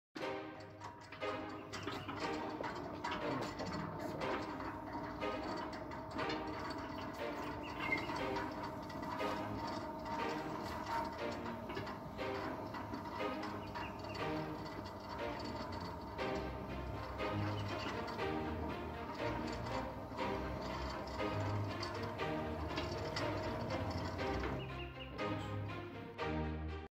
Beninca Bull 800 kg
Mr. Mazeh Project: complete the installation and operation of sliding gate motors.
Lock your home with FTC ...
FTC
keeps Going and Going...
address: صور- جل البحر- جنب مختبارات المشرق.